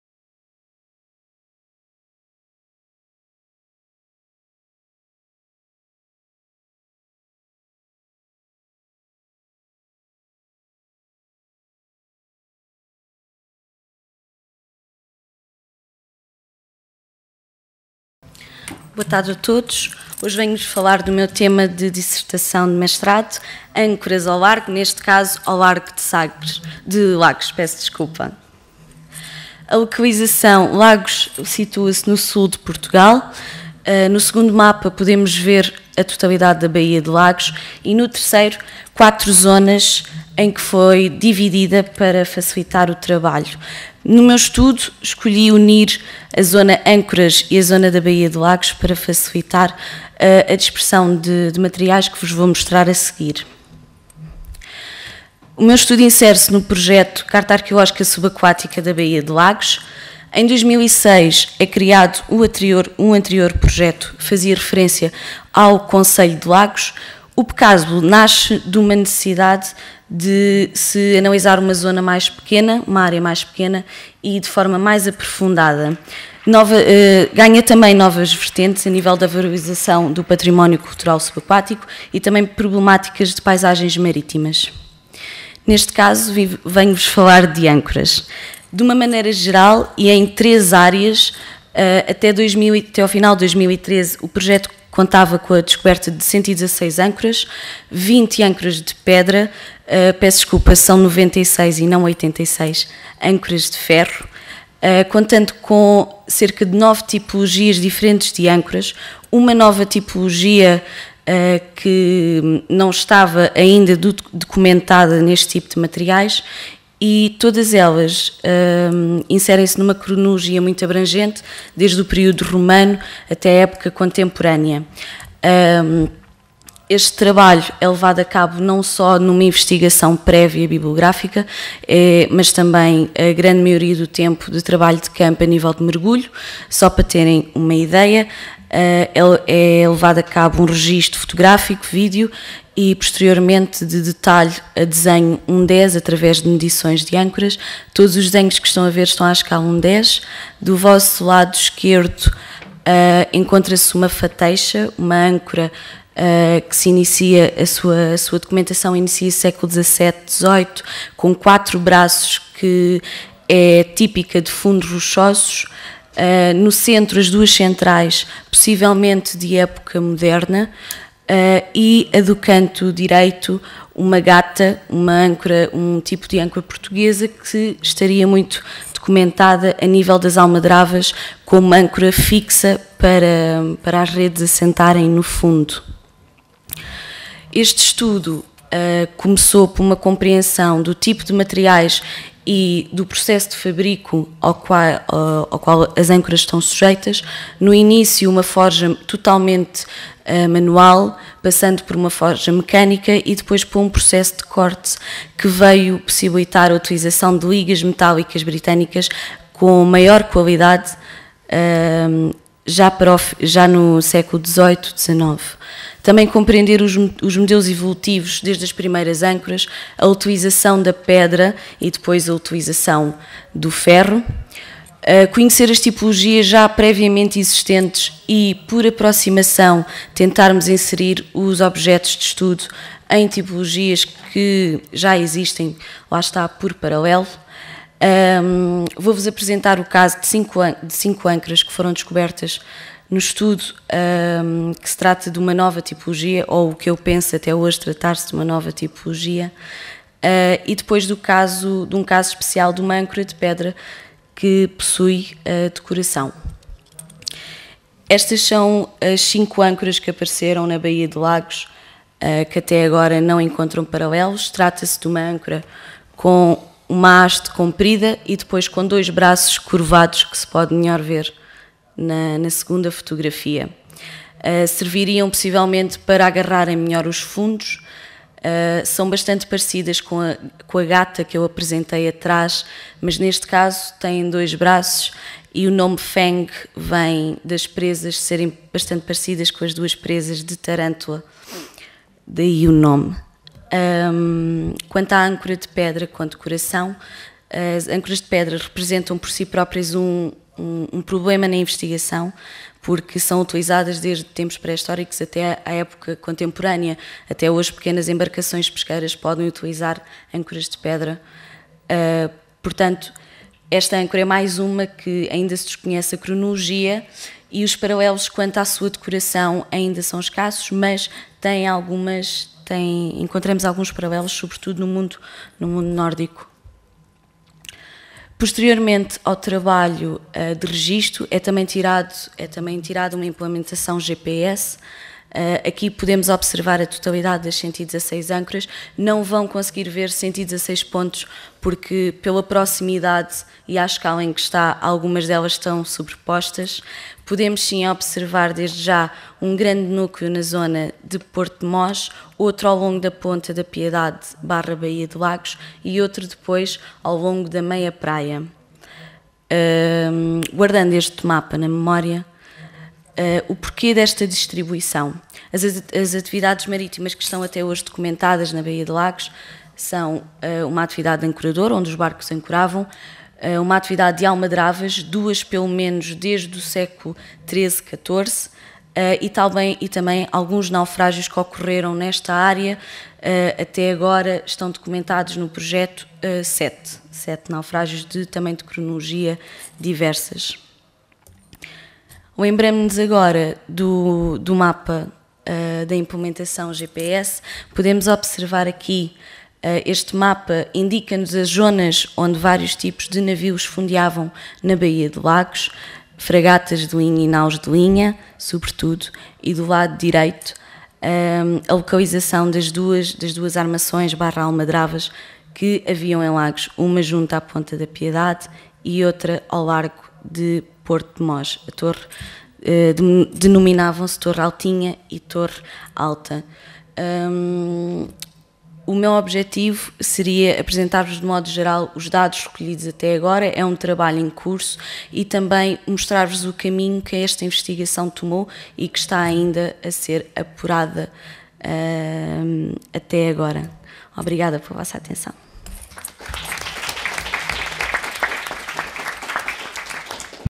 44 Âncoras ao largo: novos dados arqueológicos na Baía d
UPVEHU
CUARTA SESIÓN
Sesión Tradicional: Património cultural subaquático e sociedade: a Europa e os mares. Aula Magna, Facultad de Letras.

Copyright: Reconocimiento - NoComercial (by-nc)

Actores:

Subido por:
 ALONSO GASCA, DAVID